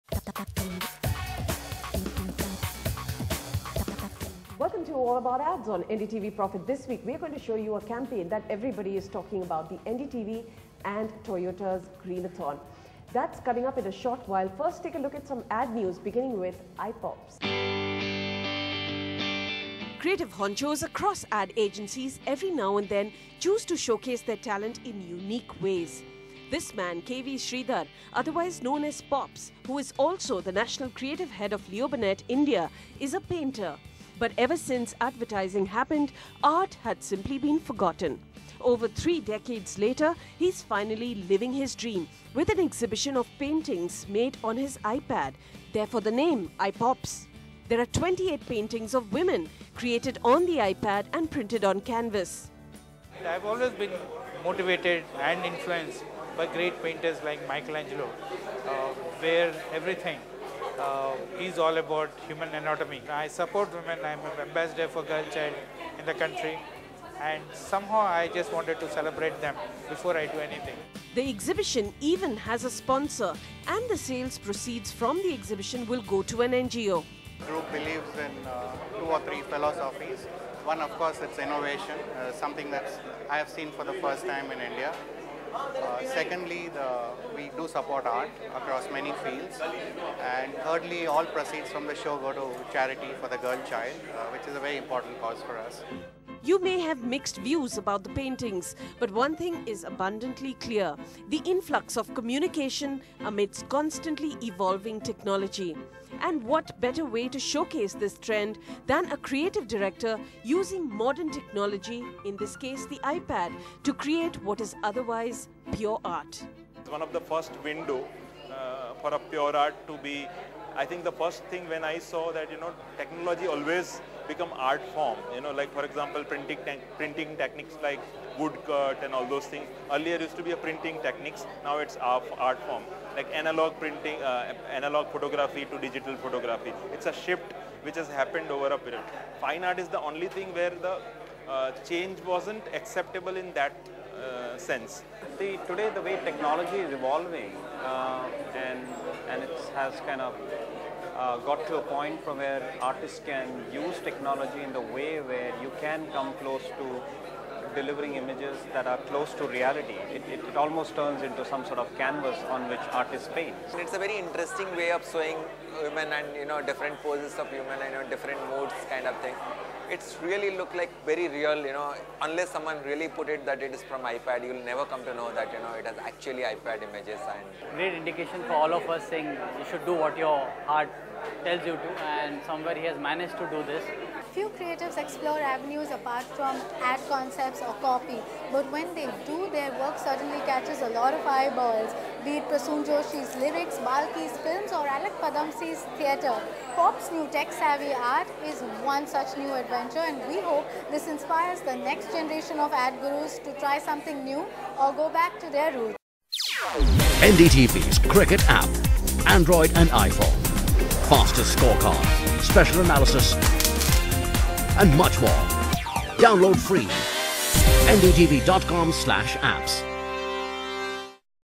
iPops -- an iPad paintings exhibition by KV Sridhar
All About Ads: K V Sridhar aka Pops, national creative director of Leo Burnett, recently showcased his collection of 28 paintings created on an iPad. The art show was held at the Scarecrow Art Gallery in Fort in Mumbai and the exhibition proceeds will go to an NGO.